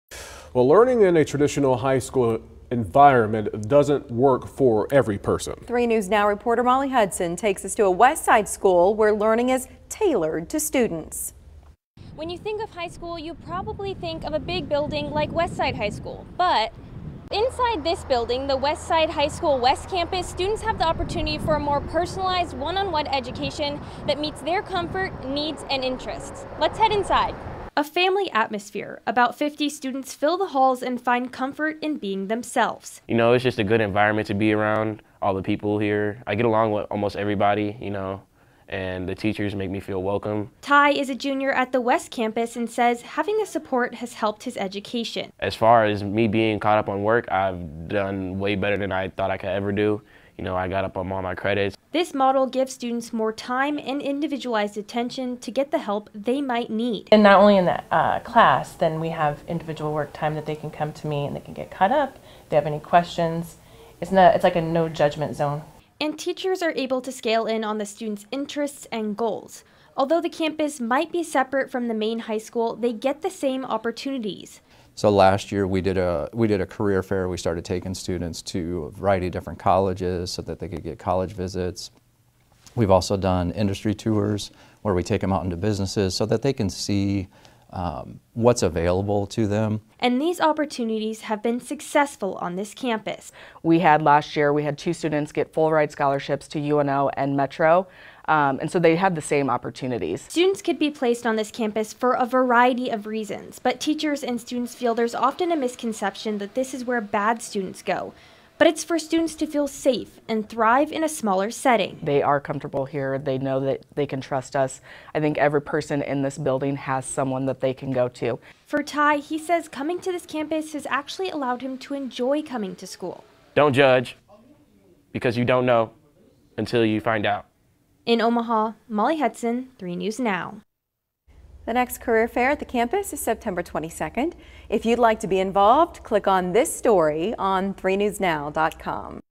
Westside High School West Campus students, teachers push back on school's reputation
Learning in a traditional high school setting doesn't work for everyone. Westside High School's West Campus focuses on tailoring learning to the students needs in a smaller setting.